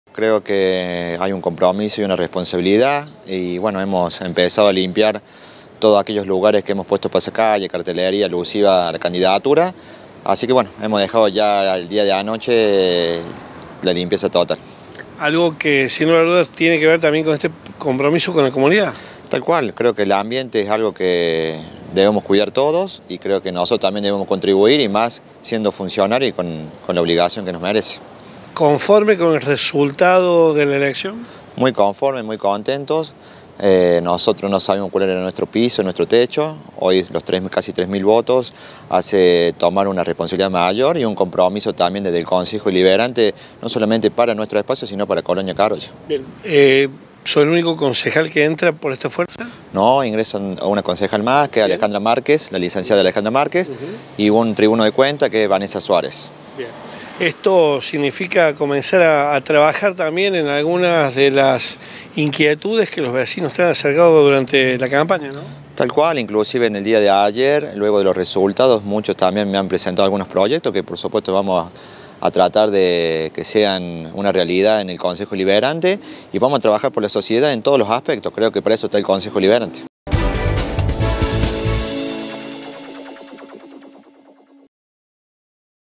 Quitaron la publicidad politica de la vía pública de Colonia Caroya
Luego de los comicios del domingo en Colonia Caroya, el equipo de Matías Roldán quitó la publicidad politica colocada en la vía pública.